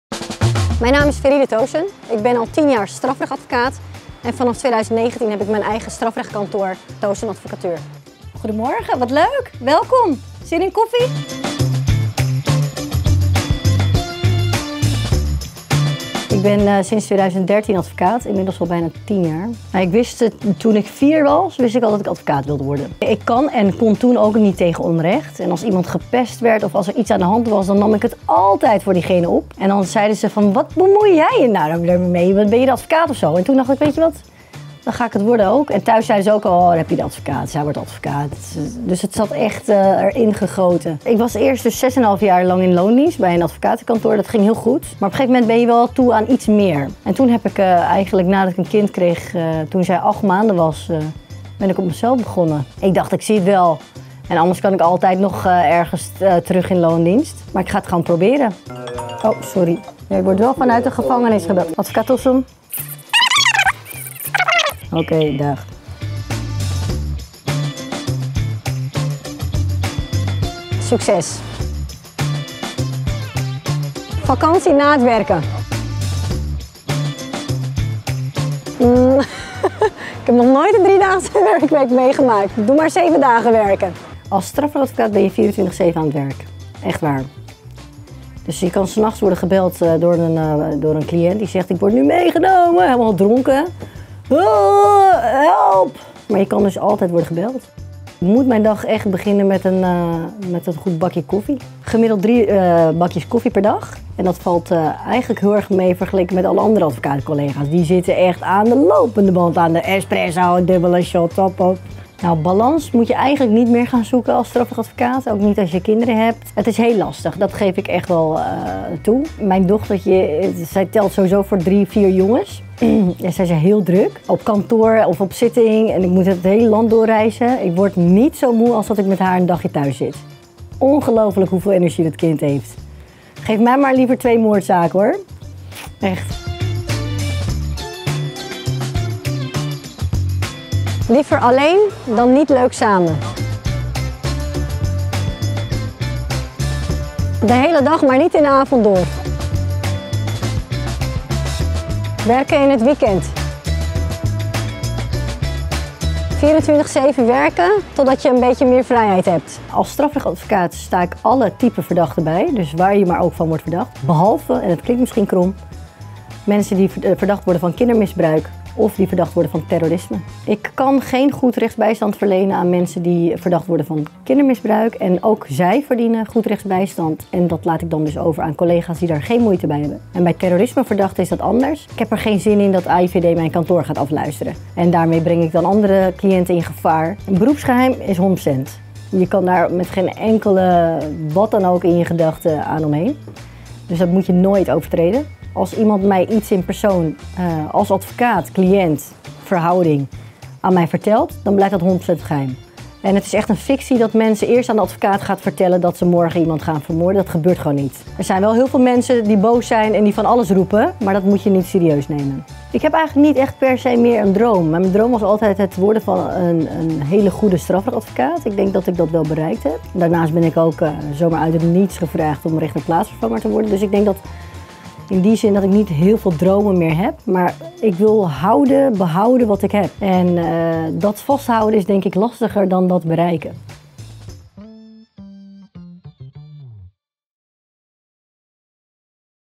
Feride Tosun over de kickstart van haar eigen advocatenkantoor - &C x Dolce Gusto
In deze eerste aflevering van de serie Kickstart nemen we een kijkje bij een van de beste strafpleiters van Nederland: Feride Tosun. Sinds 2019 heeft ze haar eigen advocatenkantoor en wij zijn benieuwd naar de kickstart van haar carrière en hoe ze zichzelf elke ochtend een kickstart geeft.